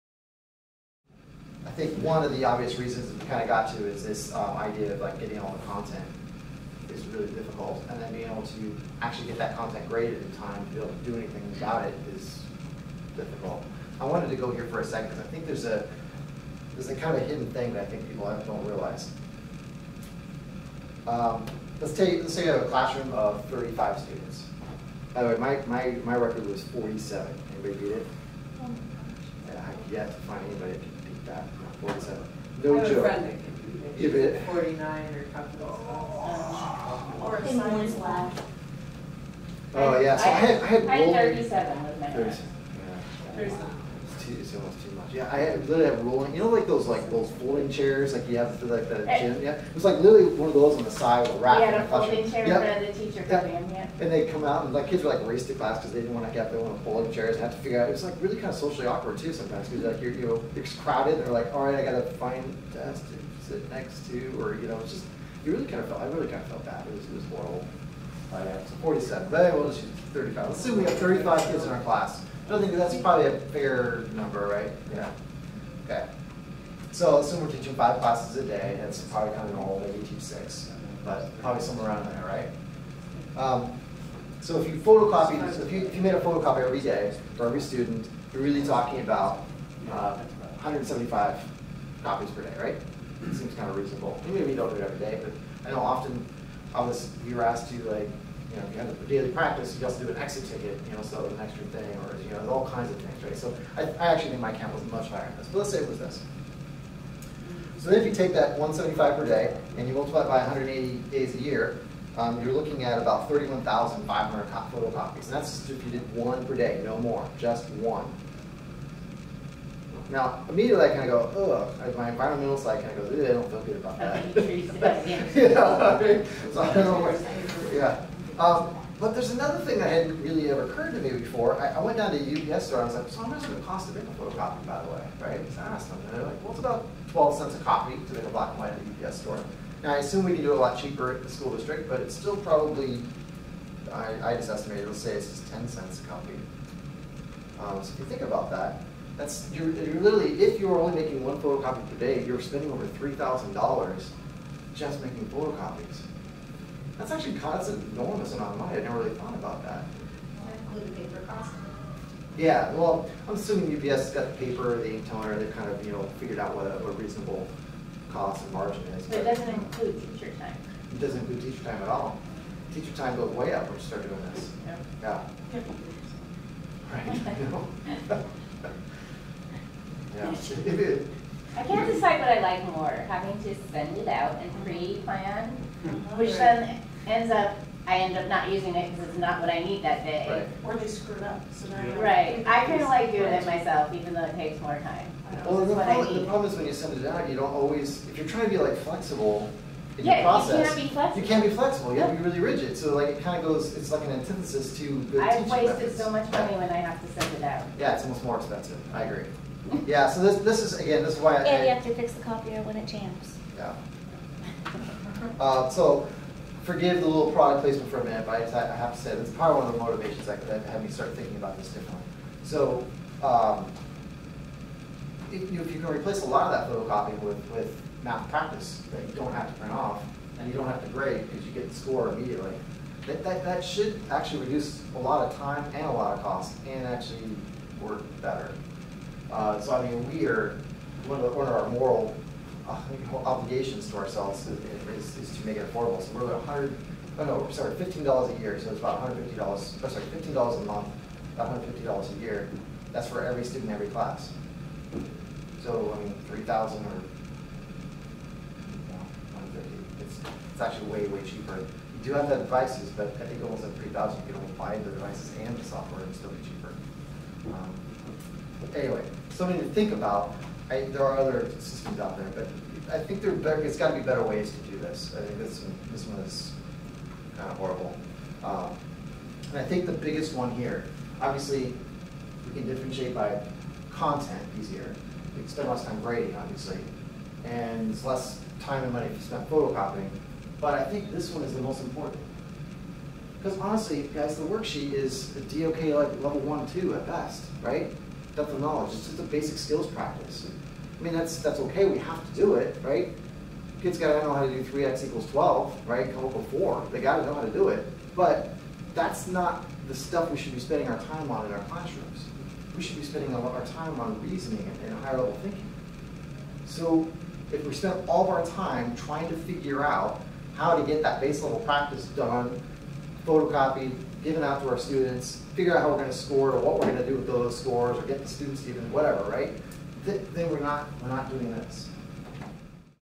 Photocopies are expensive and bad for the environment.
Willamette University: Oregon Math Leaders (OML) Conference

Software can be used to differentiate by content more cheaply than photocopies. Software can give teachers more time to differentiate by process.